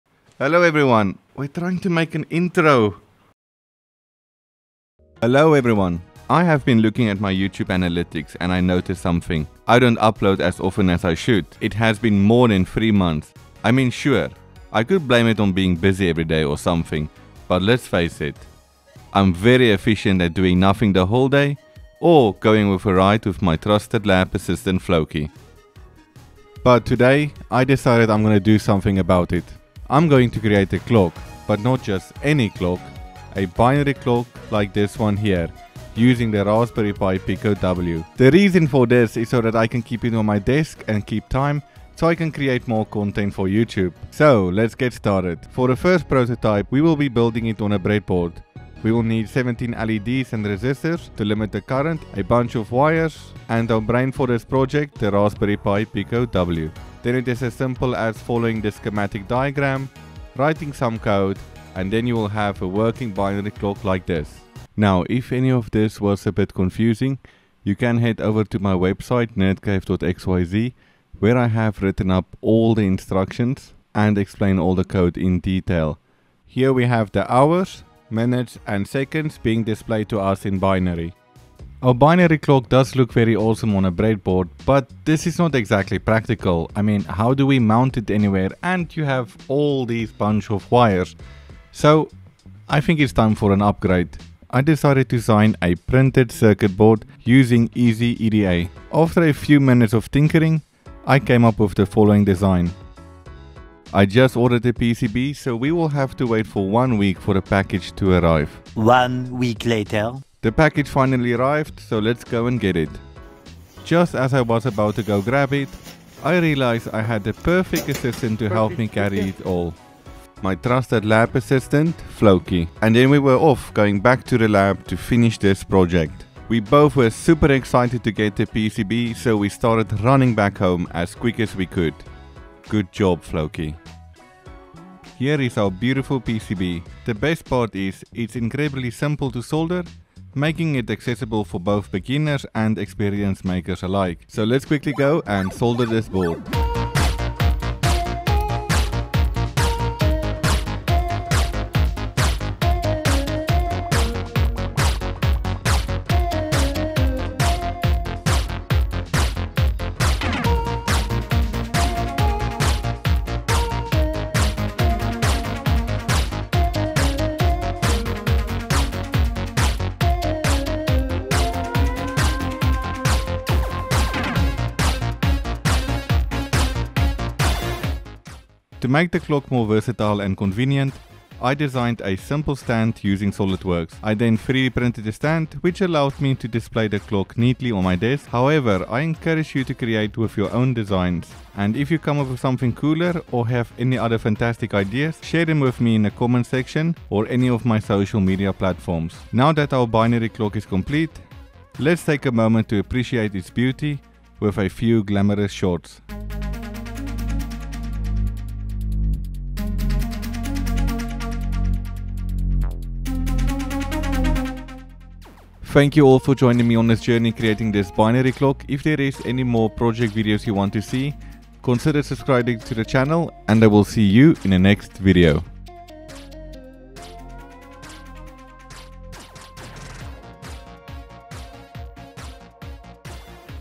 I made the Nerdiest Clock ever
Hi everyone! It's been a while, but I'm back with a different kind of video for you today. Instead of my usual tutorials, I wanted to share something special with all of you – a "how I made" video featuring none other than my adorable dog, Floki!

In this video, I'll be showcasing a fascinating project I've been working on: a Binary clock made using the Raspberry Pi Pico W. It's an exciting endeavor, and I can't wait to walk you through the process.

I'm thrilled to have you join me on this creative journey, and I hope you enjoy getting to know Floki as much as I do!